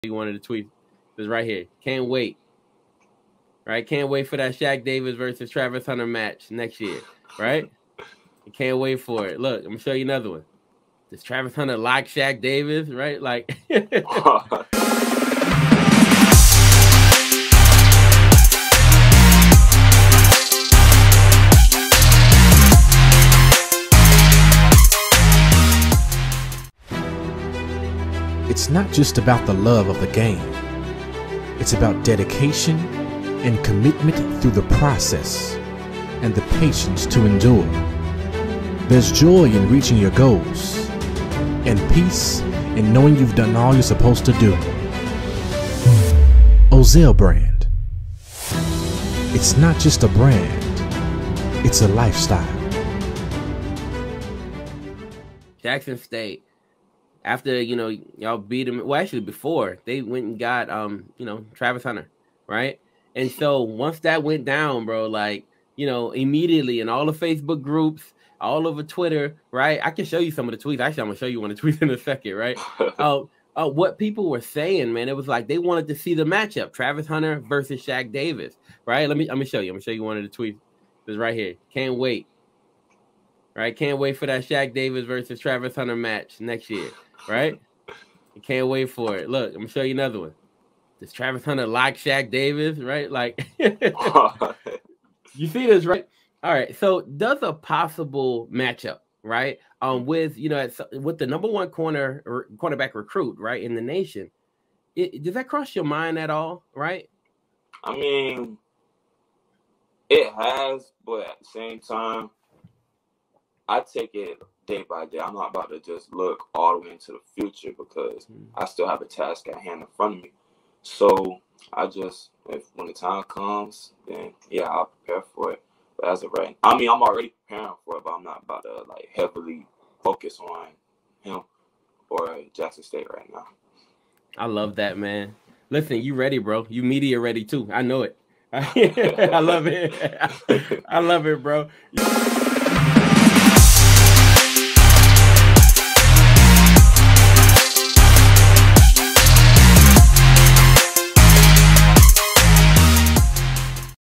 This NFL prospect could give CB Travis Hunter some problems later this season
With South Carolina State beating North Carolina Central it seems that the Bulldogs could be on their way to repeat as MEAC Champions. The Jackson State Tigers also seemed poised to make it back to Atlanta. If the two teams meet in Atlanta there will be a dream matchup that everyone wants to see. Last Year’s MVP vs the #1 Cornerback prospect in the nation last year. We spoke to the reigning MVP of the Celebration Bowl … is it on his mind and how is he preparing for the potential matchup?